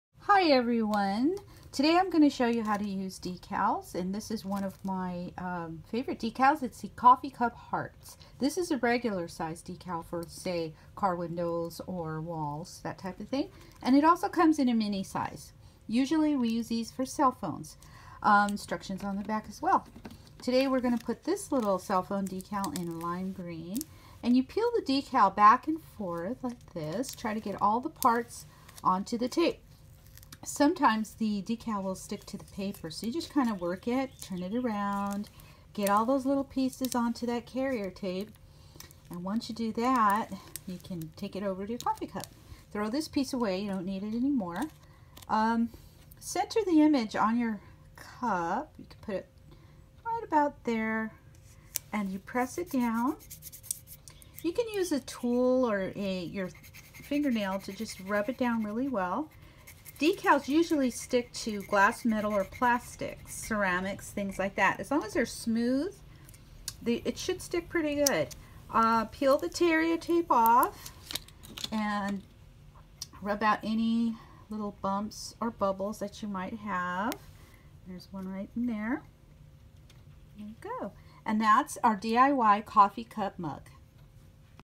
DIY Coffee Cup Decals
Aloha everyone, today I’ll be showing you how to DIY a coffee cup with vinyl decals! Decals are great to use on a variety of things. Dress up those wine glasses or give your lap top a unique style. For more DIY with decal videos join me